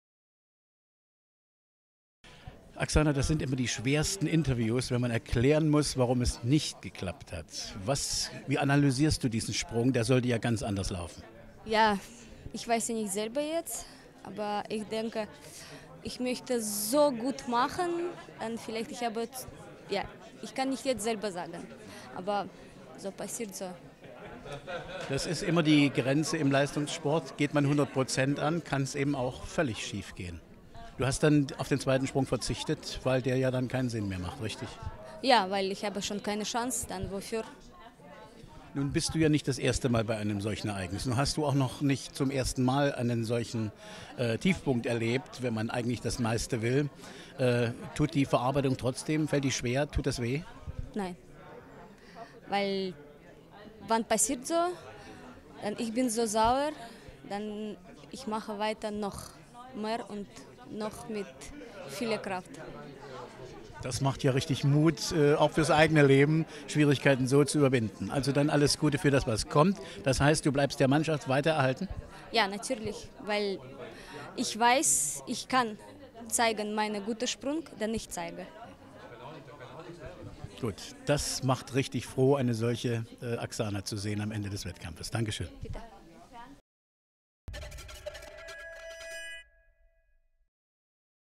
Im Interview: Oksana Chusovitina (GER)
Rotterdam, 2010, Oct. 17:
* GYMmedia-EXKLUSIV-iNTERVIEW mit der olympischen Silbermedaillengewinnerin am Sprung, Oksana Chusovitina, die hier zur WM in Rotterdam leider ihr Ziel, ins Finale zu kommen, um eine Medaille zu gewinnen, verpasste ...